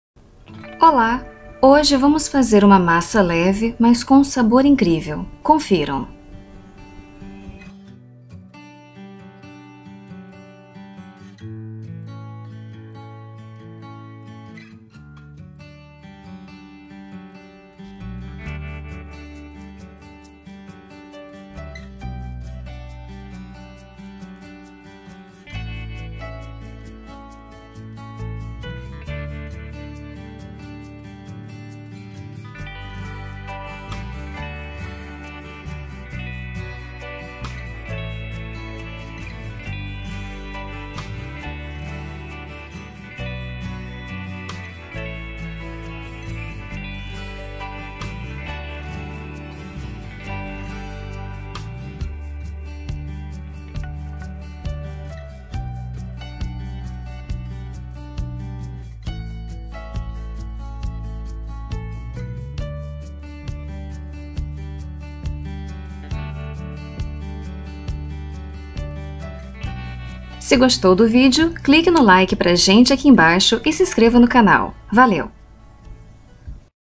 Fusilli com Sweet Grape e Manjericão
Uma receita bem simples, leve e fácil de uma massa que fica muito saborosa mesmo sendo feita com poucos ingredientes. Prática, fica pronta em pouco tempo e pode ser servida como prato único ou acompanhando sua carne favorita. Experimente!

Ingredientes e modo de preparo no vídeo acima.

Aqui, usamos fusilli (macarrão tipo parafuso), mas você pode usar sua massa favorita.

Vocês também nos encontram no facebook Delícia em Minutos e no twitter @delicia_minutos.